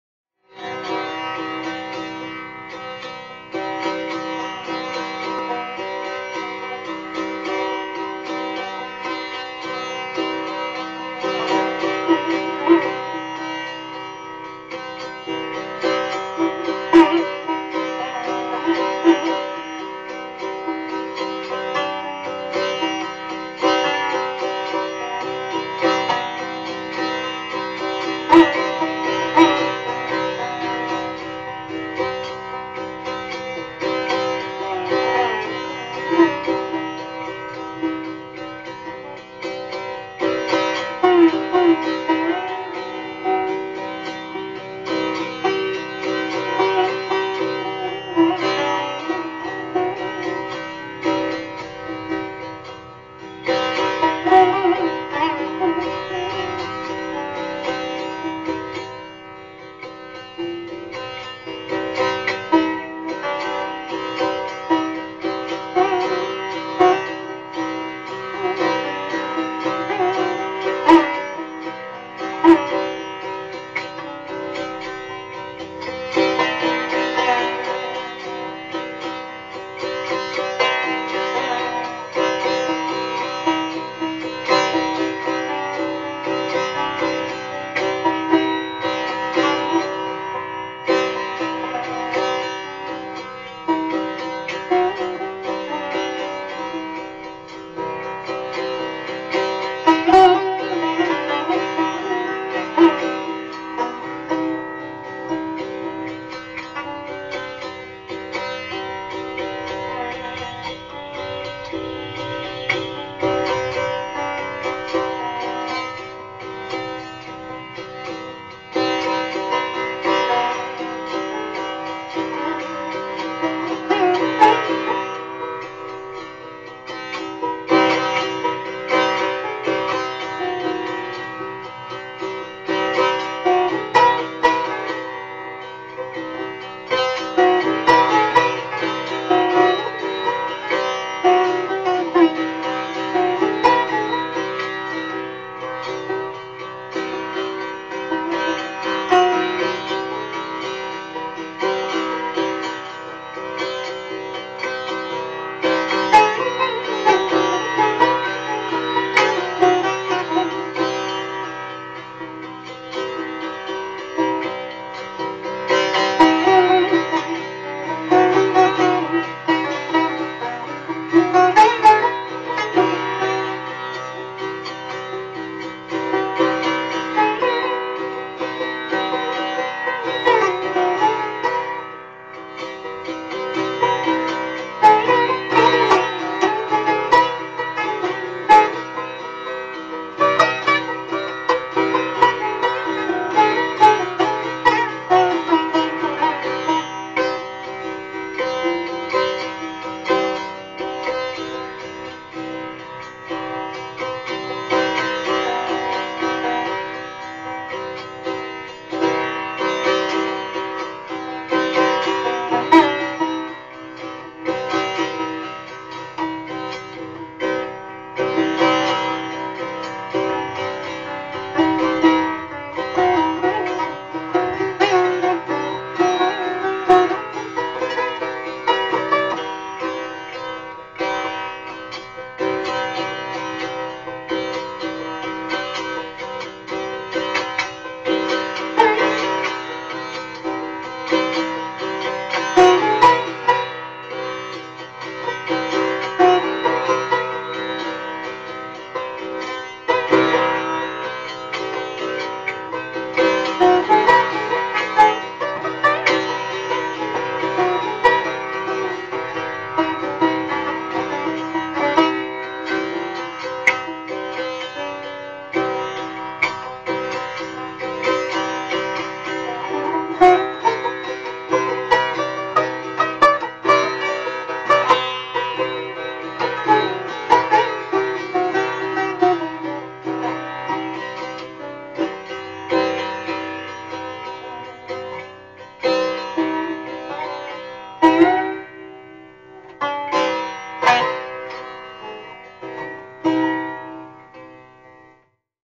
Sitar Jhala
A sitar Artist (Gum nam) Vinod Kumar Sharma Decipal of My Ustad Hashmat Alli Khan Shaib Of Ustad Vlayat Hussian Khan Shaib of Gori Pur Etiwa Retired AS Music Instructor from Institute of Music and Fine Arts Jammu At present Jammu University